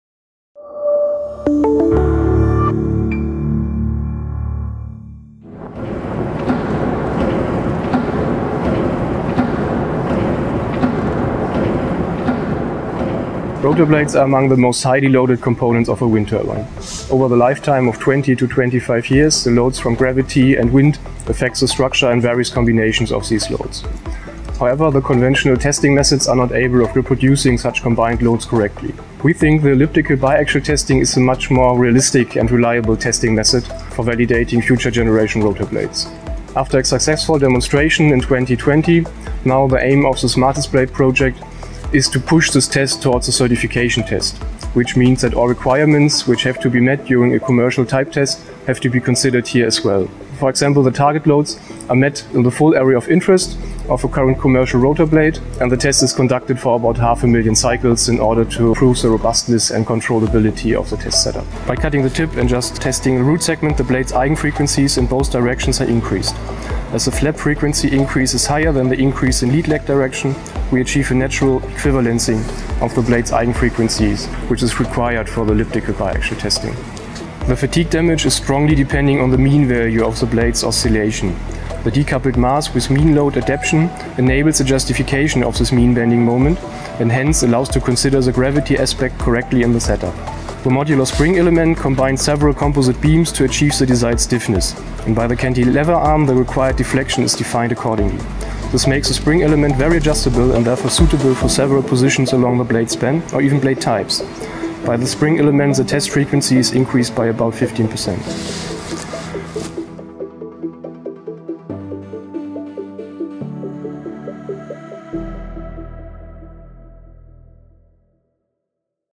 Segmented elliptical fatigue testing for future rotor blades – SmarTestBlade
Rotor blades are among the most highly loaded components in a wind turbine. Loads from wind and gravity affect the structure in a combination that cannot be reproduced by conventional testing methods.

The aim of the SmarTestBlade research project is therefore to further develop and validate methods for the realistic validation of rotor blades utilizing elliptical biaxial testing to apply combined loads on future rotor blades. This will result in a method corresponding to the requirements of a type test.

© Fraunhofer IWES/Frank S. Bauer

Segmentierte elliptische Ermüdungsprüfung für zukünftige Rotorblätter – SmarTestBlade

Rotorblätter gehören zu den am stärksten belasteten Komponenten einer Windenergieanlage. Lasten aus Wind und Schwerkraft wirken in Kombination auf die Struktur ein, die in herkömmlichen Prüfverfahren nicht reproduzierbar sind.

Ziel im Forschungsprojekt SmarTestBlade ist es, Methoden zur realitätsnahen Validierung von Rotorblättern unter Einsatz einer elliptischen Biaxialprüfung weiterzuentwickeln und zu validieren, um kombinierte Lasten auf zukünftige Rotorblätter aufzubringen. Am Ende steht eine Methode, die den Anforderungen einer Typenprüfung entspricht.